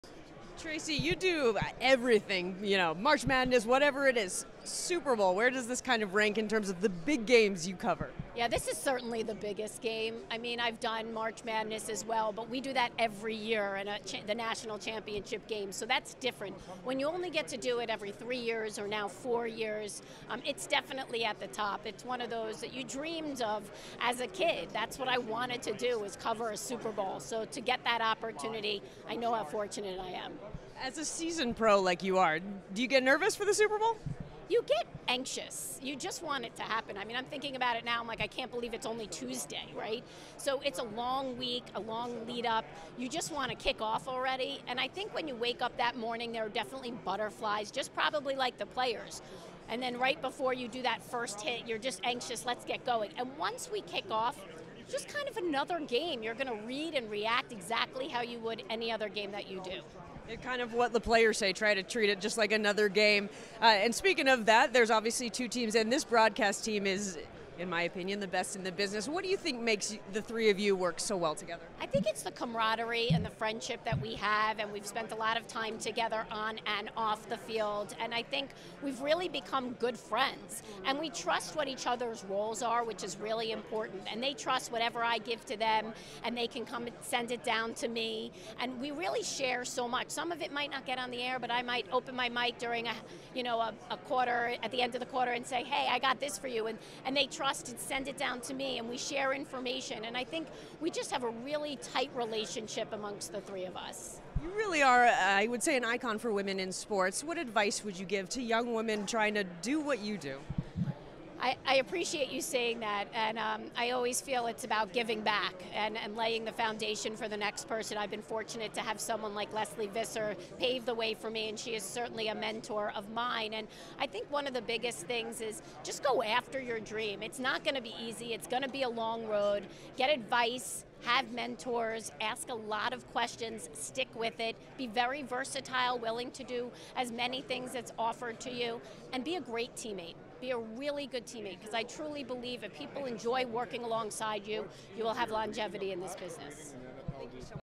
Super Bowl interview: Tracy Wolfson
Tracy Wolfson is a female mogul in sports. Why calling the Super Bowl means so much to her, what her process is like preparing for the game and advice for women looking to follow in her footsteps